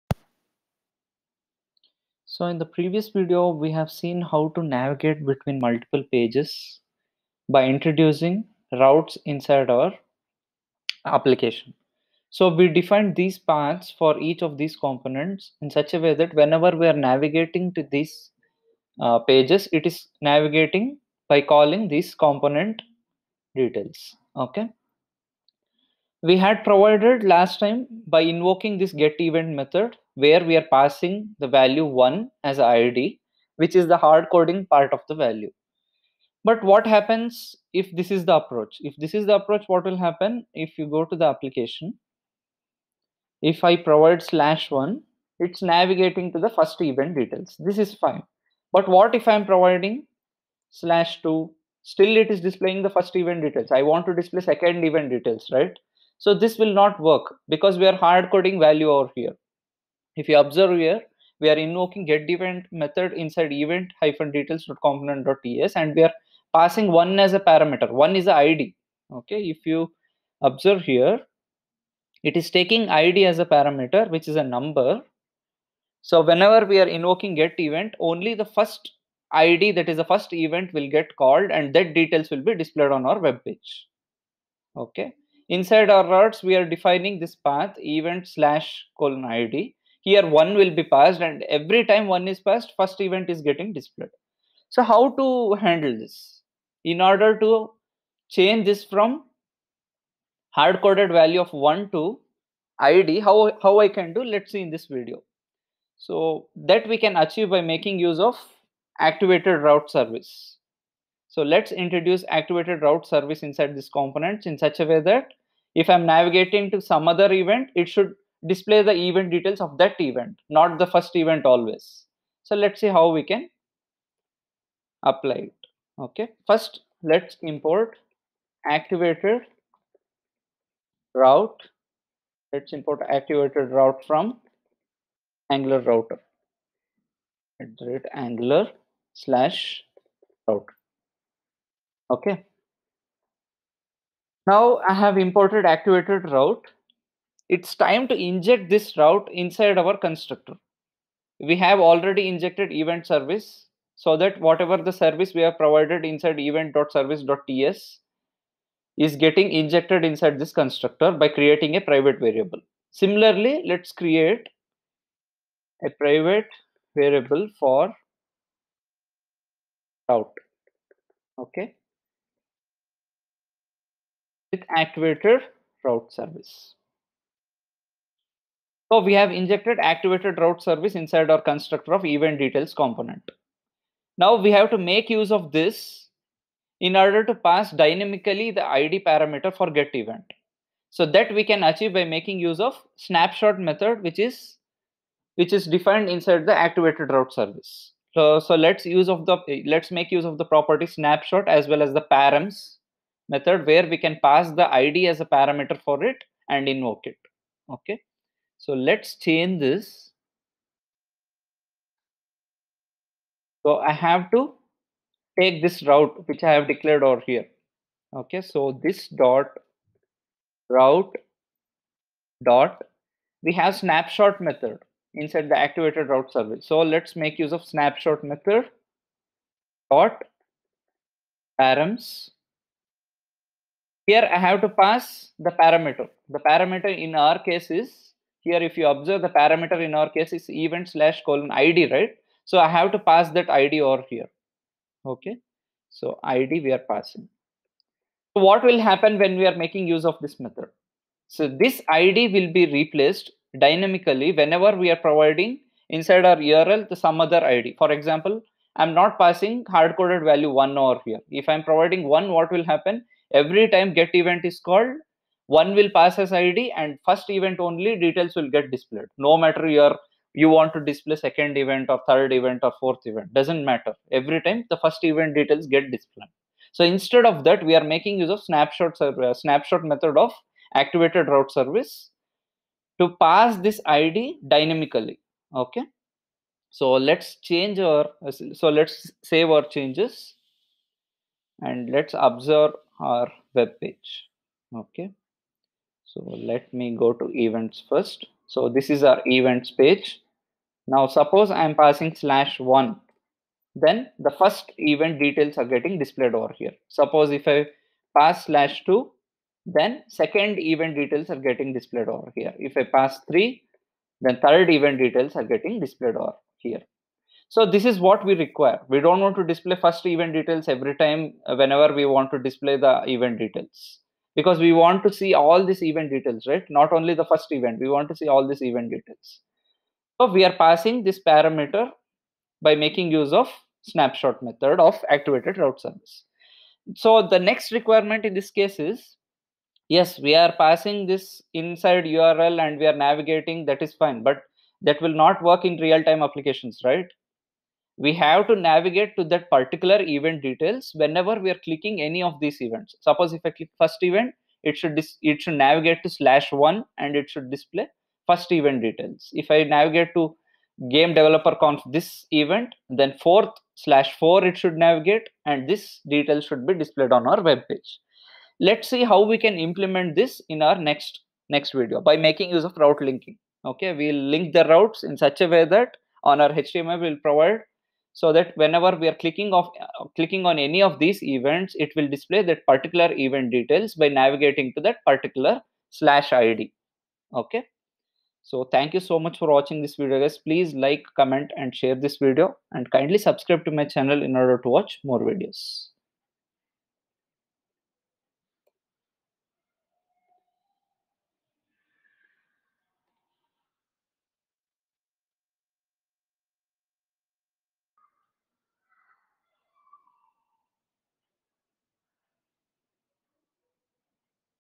66. Angular Project Tutorial - Activated Route for Event ID
66. Angular Project Tutorial - Activated Route for Event ID to take event id dynamically instead of hard coding

‎Open this link to join my WhatsApp Group for more discussions